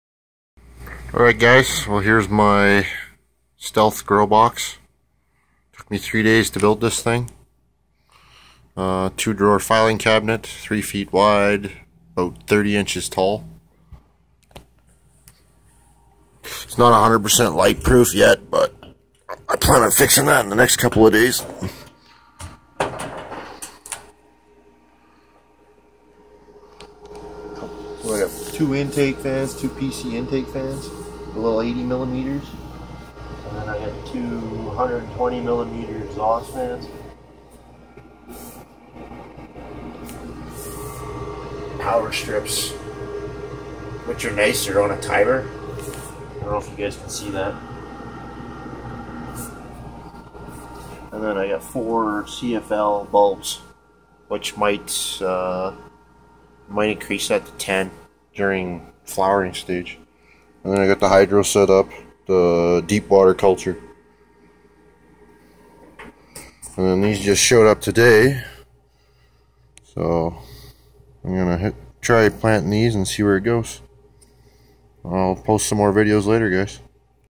Homemade Stealth Grow Box (Filing cabinet)
After construction of homemade file cabinet grow box.